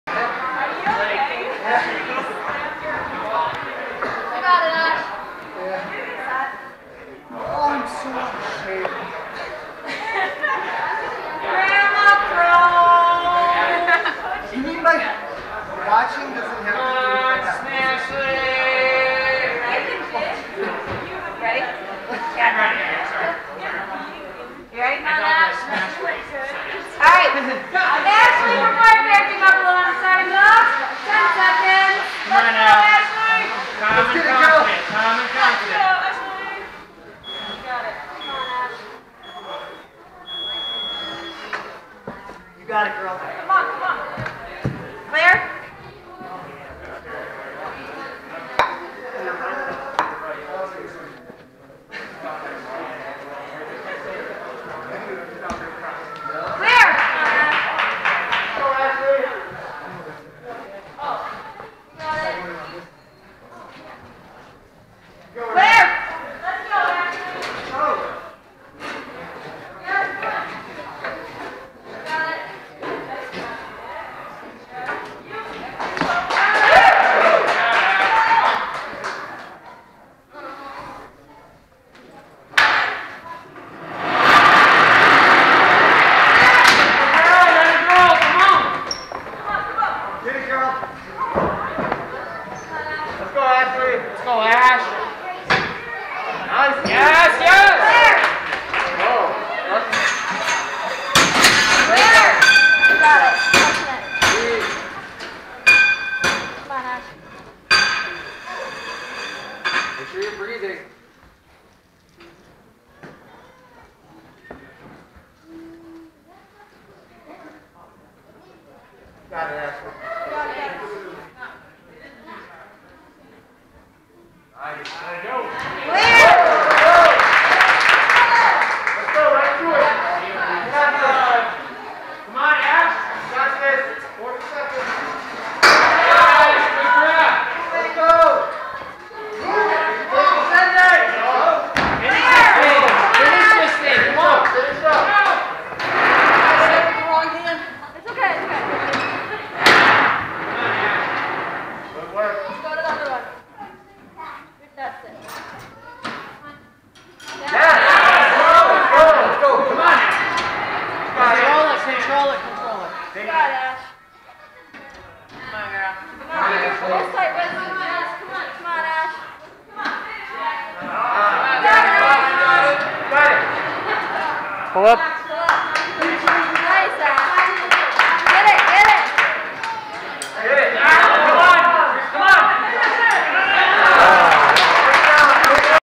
Ashley Gianni 2nd Place Build A Machine Fitness (#3) | National Ninja League Season 6
Ashley Gianni nearly finished the course.